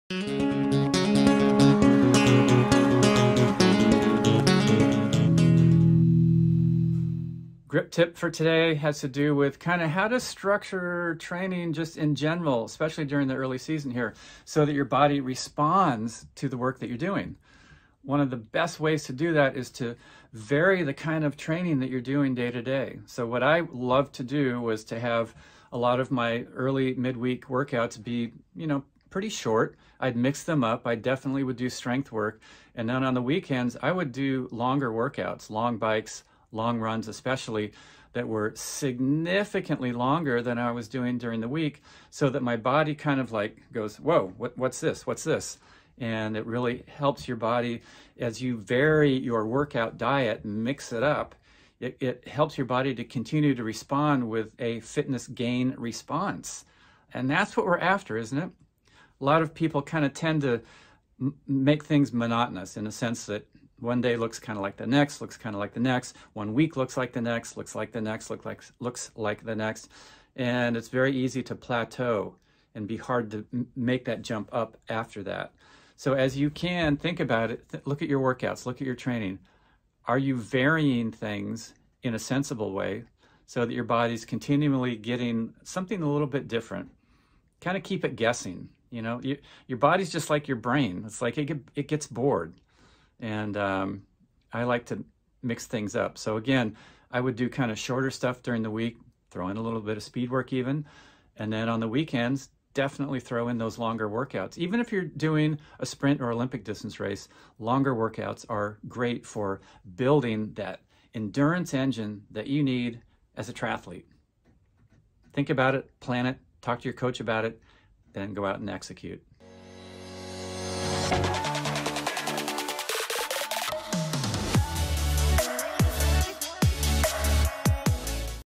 Grip Tips: Go Short Go Long
It's early in the season. Mix it up! Have each day be slightly different than the day before or the one that will follow. Go short, go long. Keep your training diet varied.